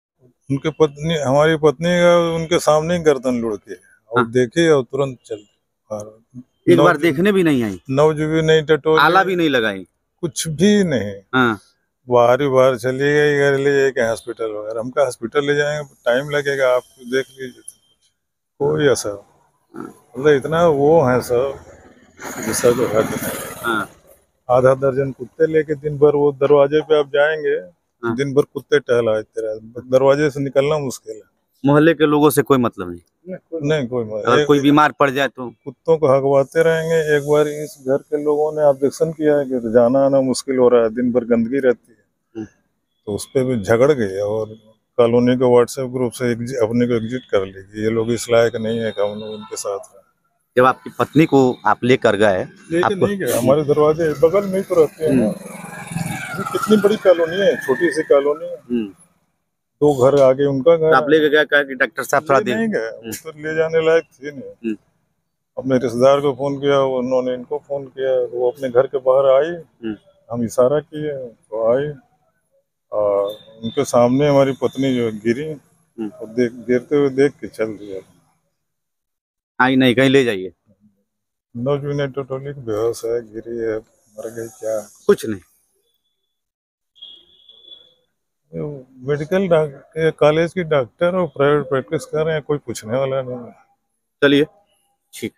डॉक्टर अनुज सरकारी के मोहल्ले वालों ने जो बताया उसको सुनकर मानवता शर्मसार हो जायेगा
गोरखपुर लाइव में आपका स्वागत है।

Gorakhpur Live